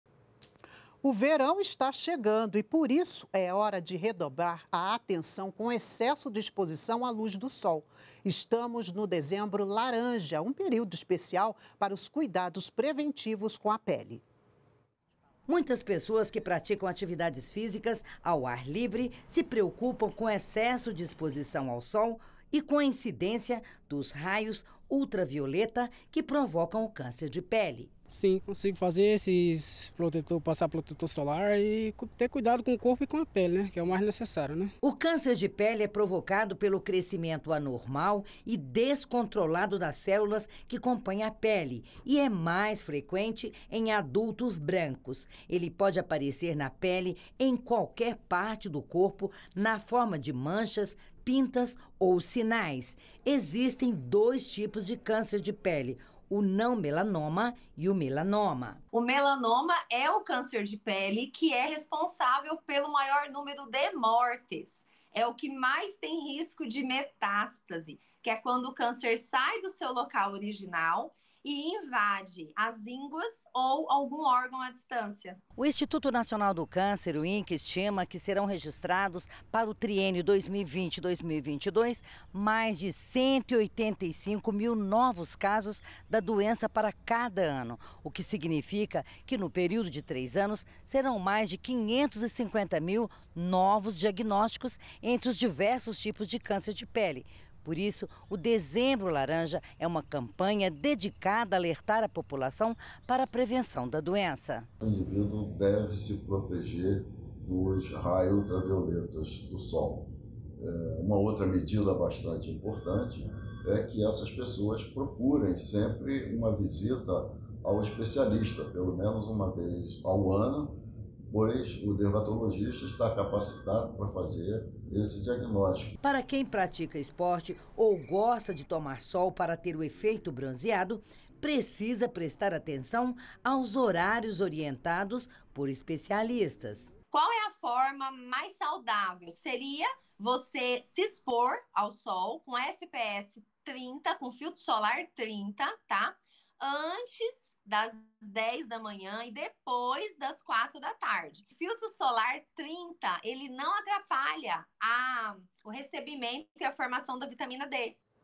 Saúde | Dezembro Laranja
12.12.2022 - Dezembro Laranja alerta para os cuidados preventivos com a pele.